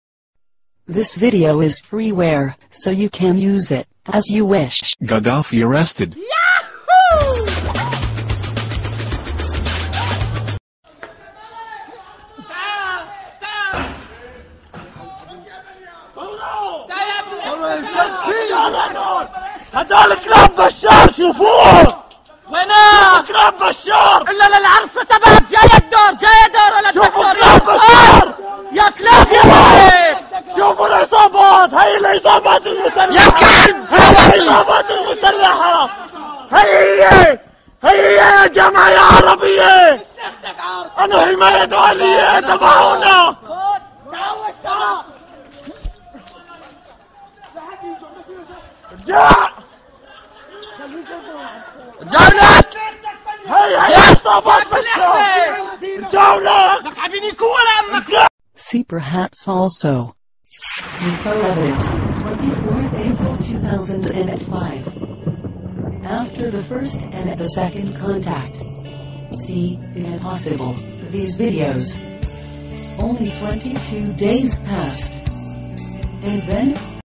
حماه | الصابونية -اطلاق رصاص على المتظاهرين 28/10 +
standard YouTube-licens
to ...
Creative Commons-License

So ...

8 ...

Internal system-reference:
Luke 14,17

The searchstring was:
فري برس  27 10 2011

to English:
Free Press, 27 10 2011

Unedited headline translation:
Hama | soap - shooting at the demonstrators 28/10